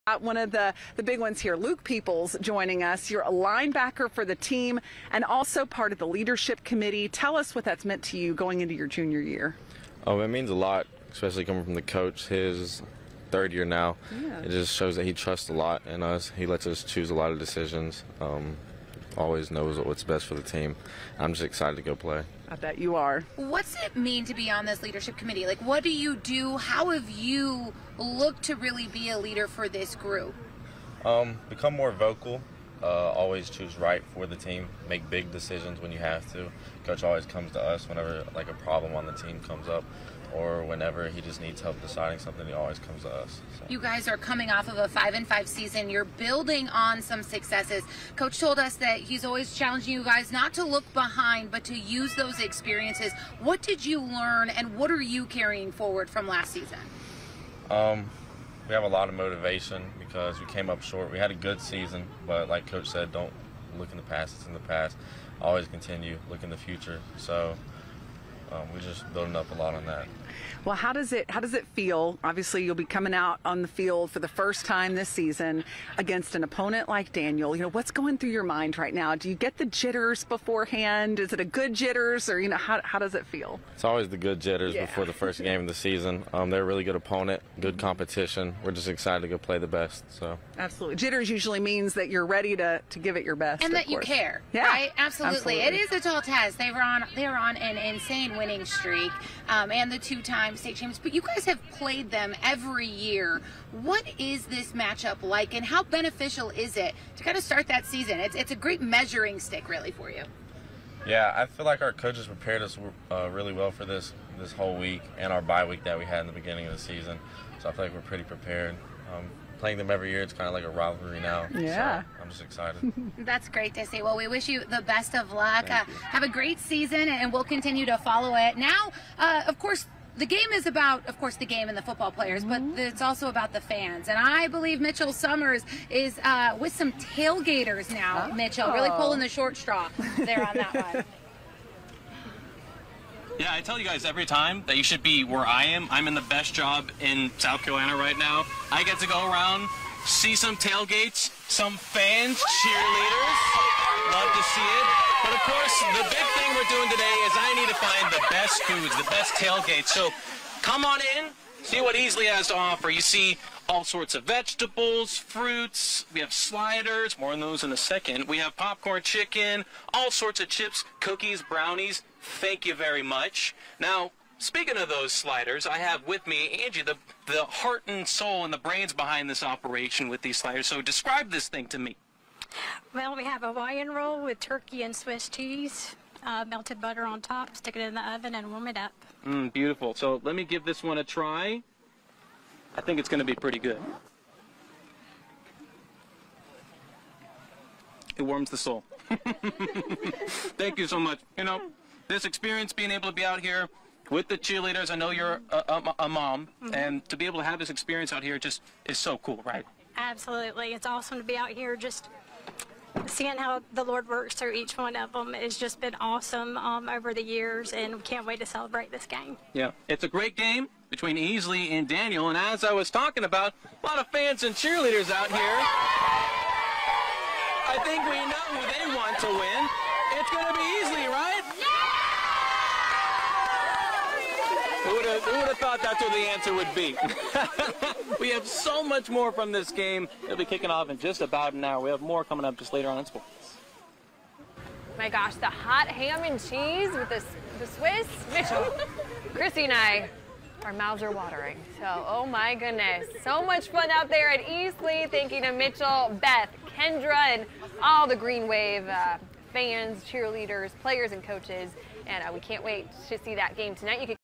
Easley football player talks about the upcoming game
Kendra Kent and Beth Hoole speaks with Easley football player ahead of the game against Daniel.